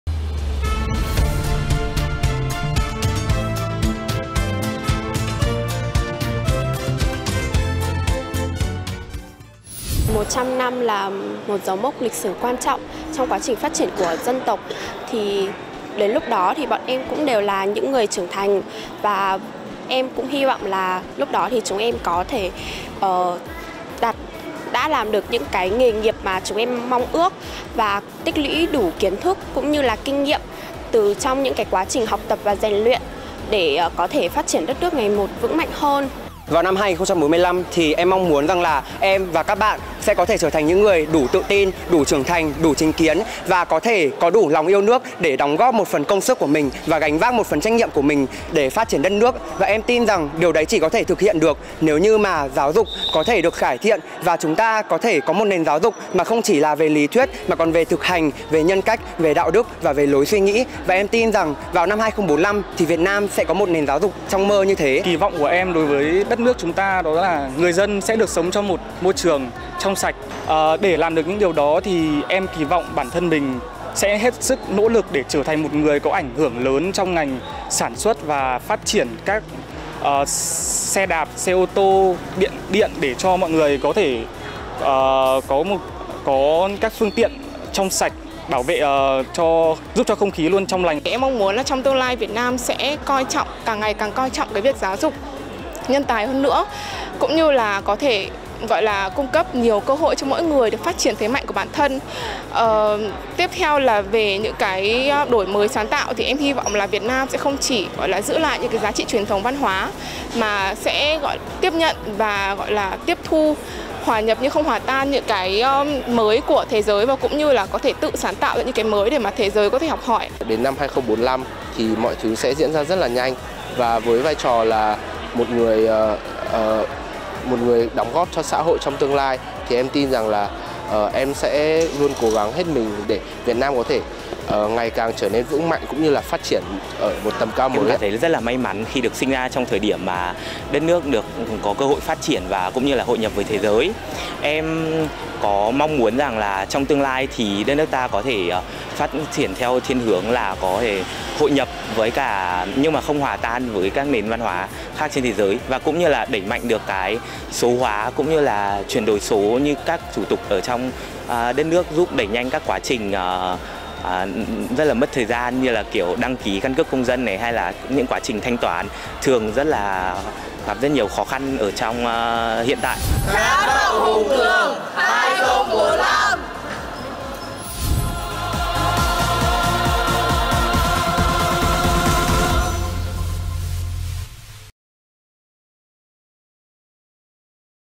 Học sinh Trường THPT Chuyên KHTN nói về khát vọng 2045
“Khát vọng cống hiến – Lẽ sống thanh niên” là chủ đề đợt sinh hoạt chính trị nhằm góp phần giáo dục thanh niên tự rèn luyện nâng cao đạo đức Cách mạng, loại bỏ chủ nghĩa cá nhân để trở thành người công dân có ích cho xã hội; nêu cao khát vọng cống hiến cho Đảng, cho Tổ quốc, cho Nhân dân, đồng thời coi đó là lẽ sống của mỗi người trẻ.

 Ngày 30/11/2022, tại Hội trường Ký túc xá Mễ Trì (182 Lương Thế Vinh, Hà Nội), Trung ương Đoàn TNCS Hồ Chí Minh phối hợp với Trường THPT Chuyên KHTN đã tổ chức chương trình Giao lưu Khát vọng 2045. Đây là hoạt động giao lưu ý nghĩa giữa các học sinh THPT tiêu biểu về những hành trang, những khát vọng, ước mơ của các bạn trẻ cho thời ngưỡng lịch sử 2045.

Trong quá trình thực hiện chương trình, ekip thực hiện cũng đã tổ chức khảo sát và lấy ý kiến một số em học sinh Trường THPT Chuyên KHTN về khát vọng 2045 – Những ước mơ và mong muốn của chúng ta.